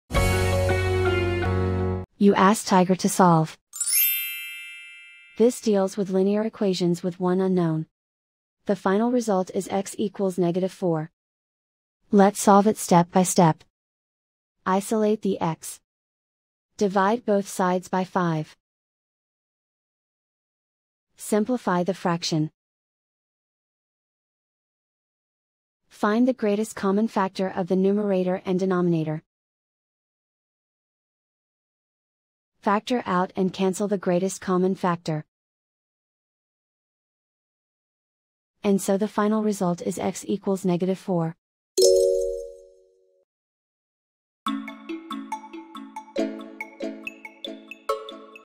Solve 5x=-20: Linear Equation Video Solution | Tiger Algebra
Struggling with 5x=-20? Watch our step-by-step video on Tiger Algebra to master this linear equation effortlessly.